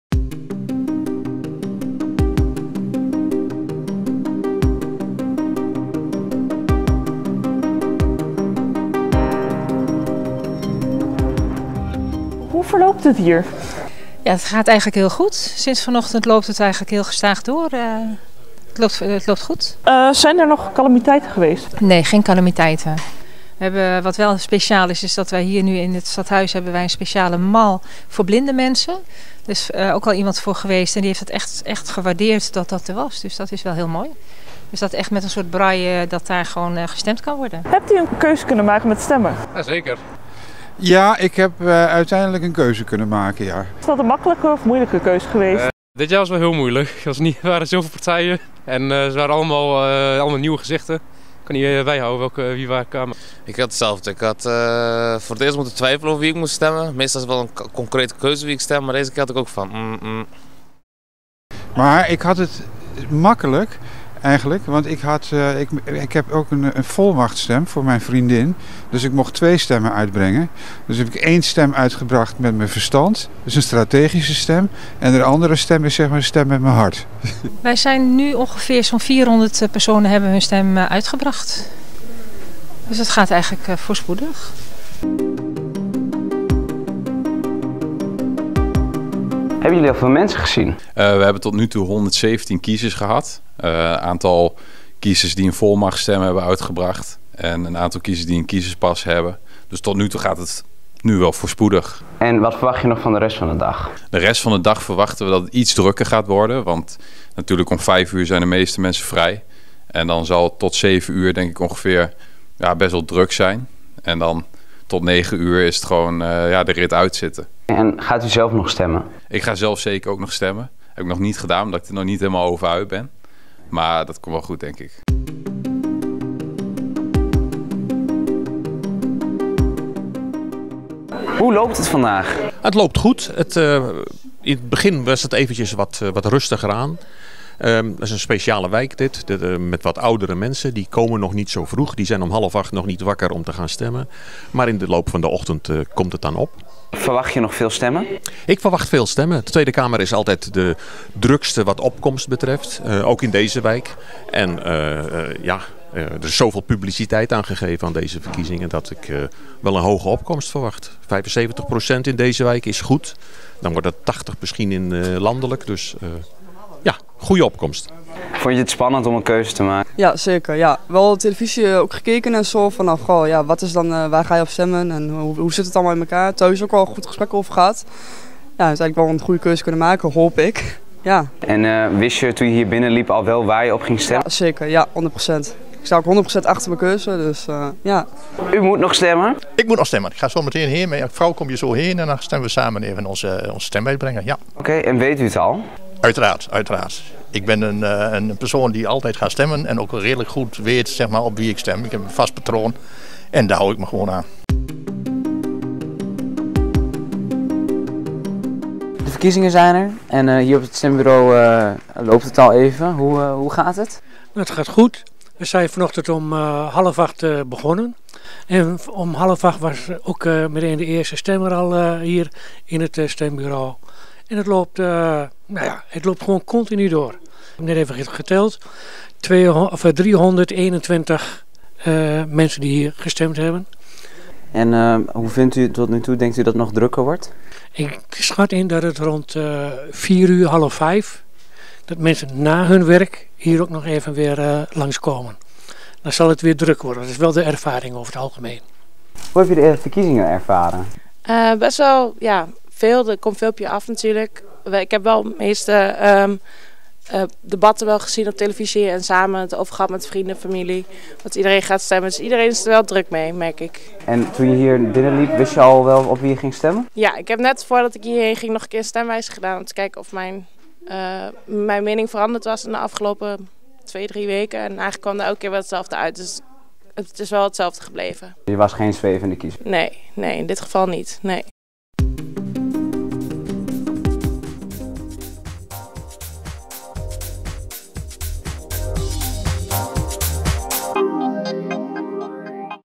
Verkiezingsdag in Almelo
Heel Nederland staat vandaag in het teken van de verkiezingen. Iedereen boven de 18jaar mag zijn of haar stem uit brengen op een van de 26 partijen die meedoen aan de Tweede Kamerverkiezingen.